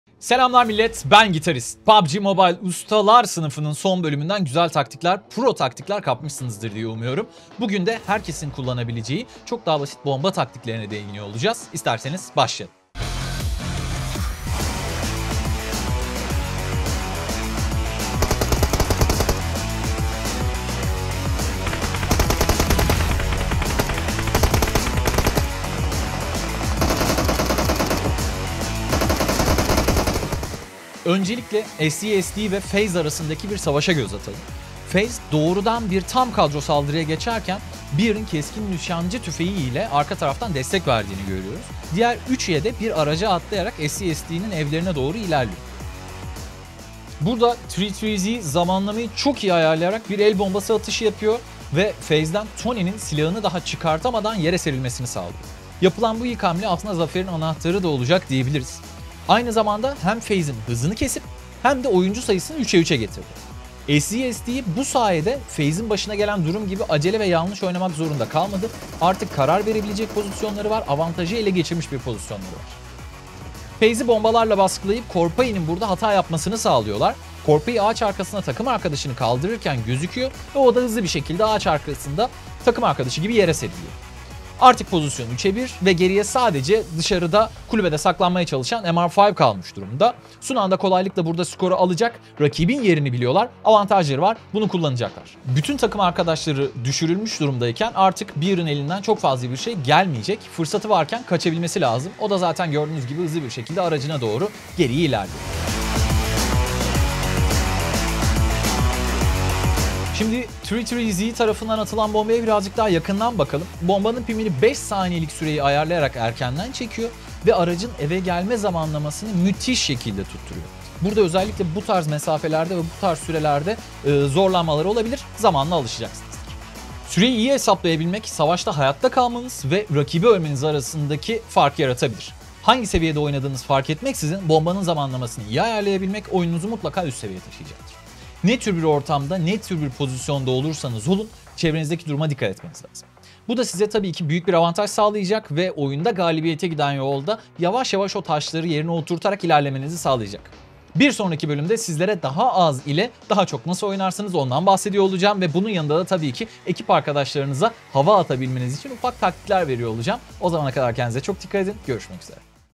PMGC Usta Sınıfı - Bölüm 2: Rakip Takımın Oyunu Nasıl Bozulur?
Oyunda iyi olmak için profesyonel oyunculardan taktikler almak ister misiniz? O zaman hazır olun!

PMGC Usta Sınıfı’nı izleyin ve oyunu ustalarından öğrenin, alanın tozunu attırın!

Bu bölümde bomba kullanımının önemini, zamanlamanın önemini, ve çevre kontrolünün önemini işledik. Sonraki bölümler için yorumlarınızı ve önerilerinizi şimdiden merak ediyoruz... 🤔